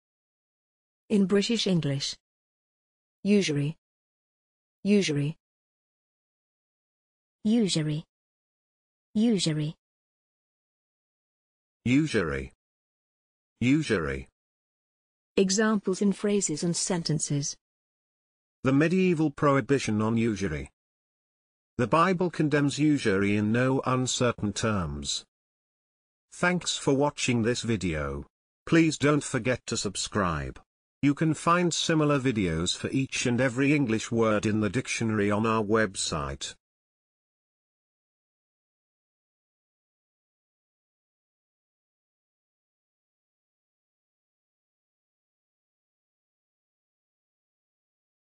usury - pronunciation + Examples in sentences and phrases
This is the pronunciation of usury in British English + a few examples of its pronunciation in phrases and sentences.